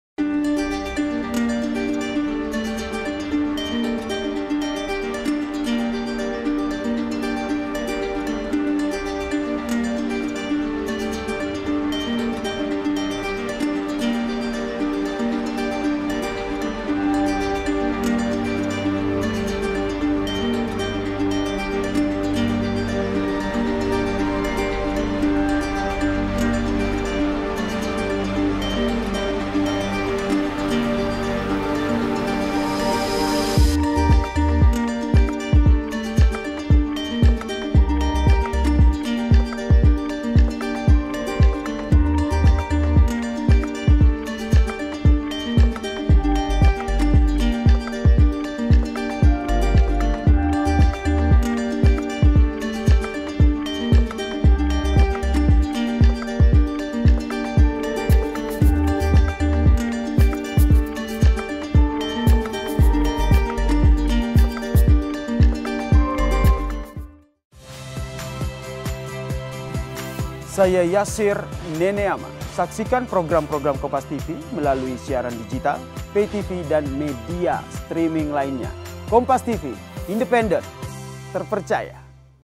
Potret Akrab Jokowi Bersama Putin di Beijing
KOMPAS.TV - Potret Akrab Presiden Jokowi Bersama Presiden Rusia Vladimir Putin di Beijing